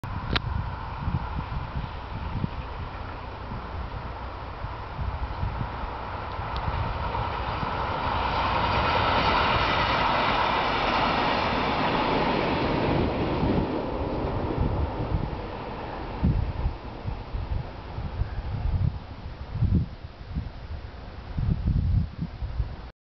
IC912(Tournai-Liers)à Jemeppe/s/Sambre (L130) 21/08/2009, 14h19
IC912 (Tournai - Liers) circule ligne 130 à l'entrée de Jemeppe-sur-Sambre.